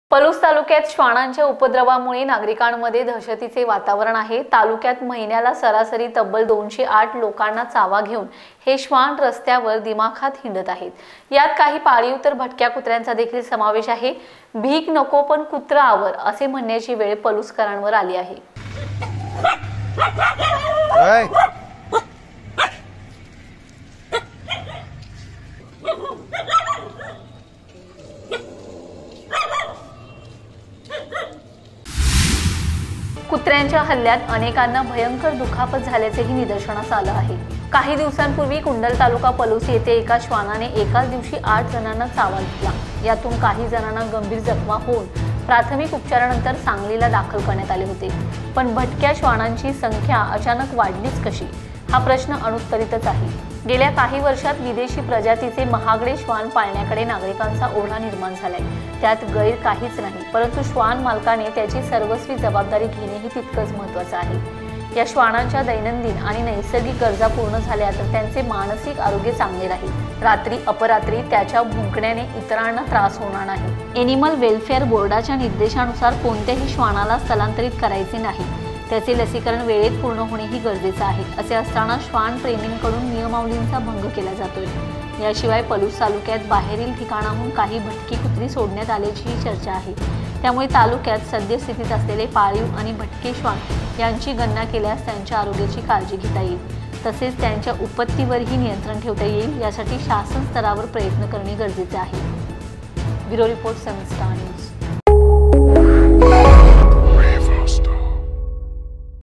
सावधान...! पलूसमध्ये येताय, मग ही कुत्री तुम्हाला धरणार?
कुत्र्यांच्या चाव्याने तब्बल ८३१ जण जखमी